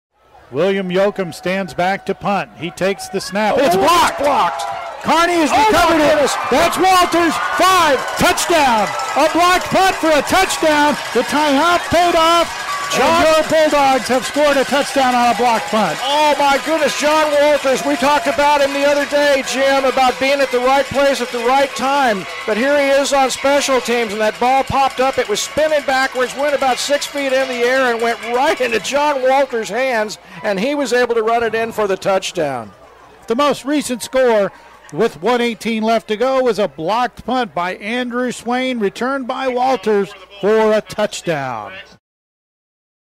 Kearney Chamber Play of the Week
Have a listen to this blocked punt returned for a Kearney Bulldogs touchdown.